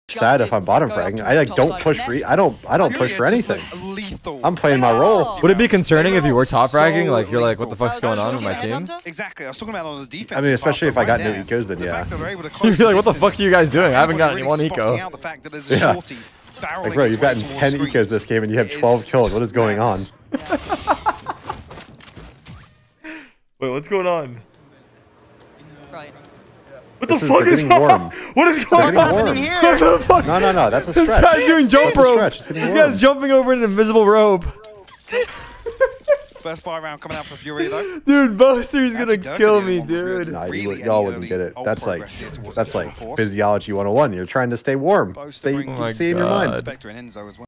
"Boaster's gonna kill me dude" 🤣 | SEN Tarik's reaction to Boaster doing invisible jump rope
ignore

Tarik compilations